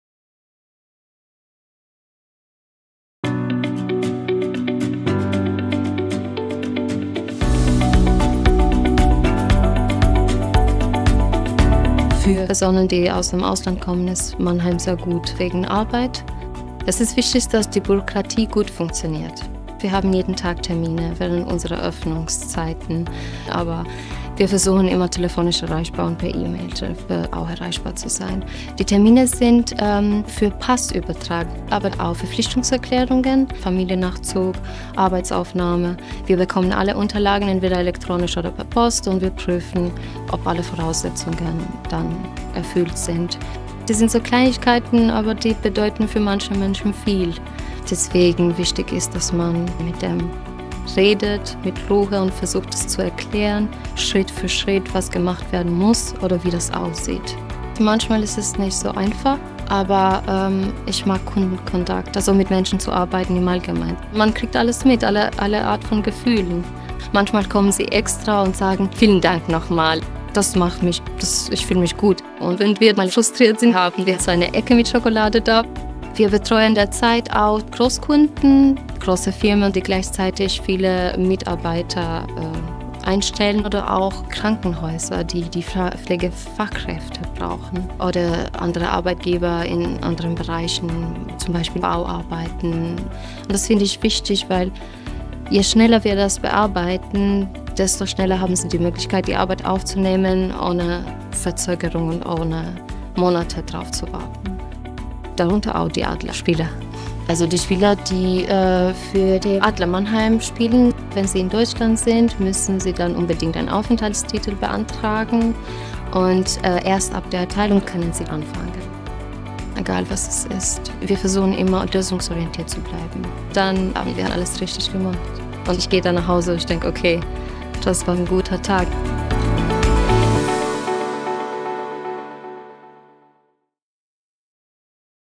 Arbeiten bei der Ausländerbehörde Mannheim
Kostanca R. ist Sachbearbeiterin bei der Ausländerbehörde Mannheim, die sich Weltoffenheit zur Mission gemacht hat.